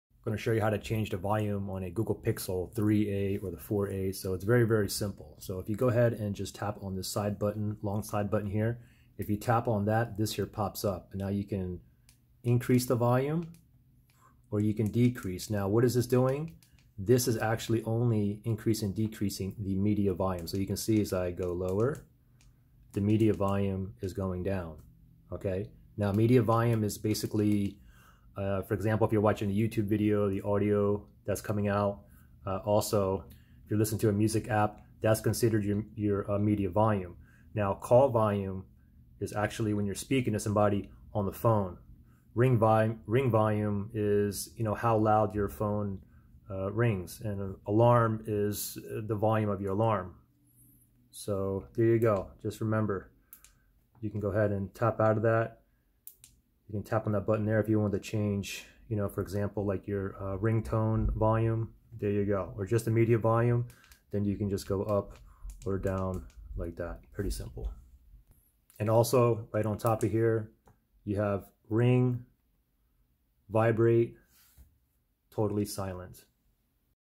Pixel 3a/4a: How to Adjust Volume (Media/Call/Ring/Alarm Volume)
I show you how to change or adjust the media, call, ring and alarm volume on the Google Pixel 3a or 4a. Hope this helps.

Google Pixel 4a - New Unlocked Android Smartphone